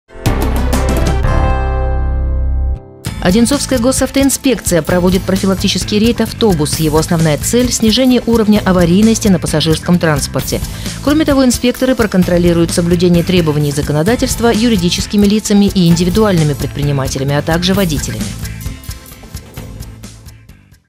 Профилактический рейд «Автобус»
Одинцовская Госавтоинспекция проводит профилактический рейд «Автобус». Его основная цель — снижение уровня аварийности на пассажирском транспорте.